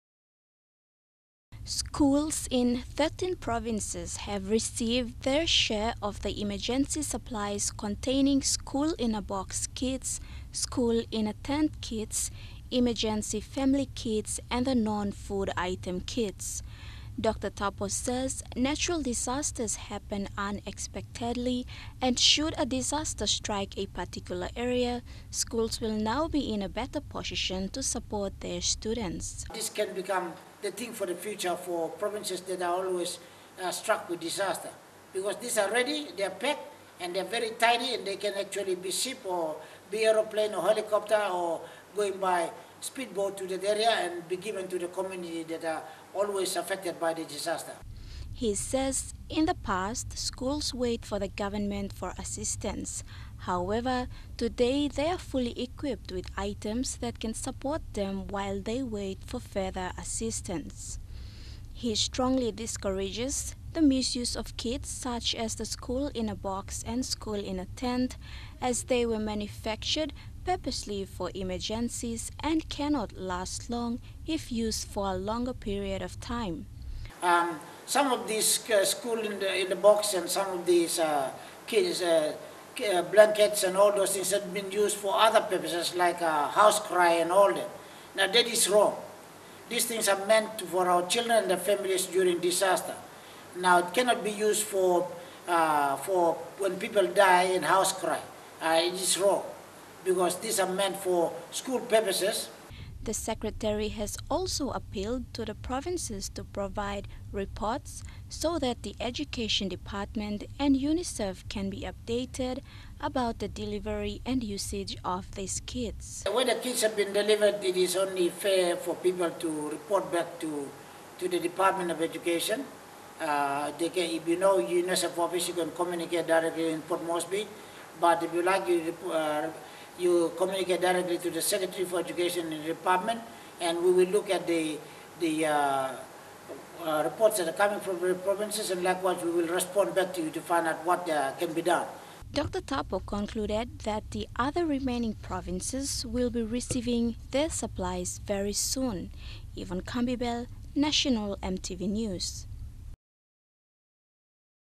Schools Urged to use Emergency Kits
Schools around the country are being encouraged to utilise emergency kits being supplied to them by the Education department and UNICEF.

Education Secretary, Dr Michael Tapo said last week, that the purpose of the emergency supply kits are to help support students and their families, and must be used correctly whenever a natural disaster strikes. for the latest news...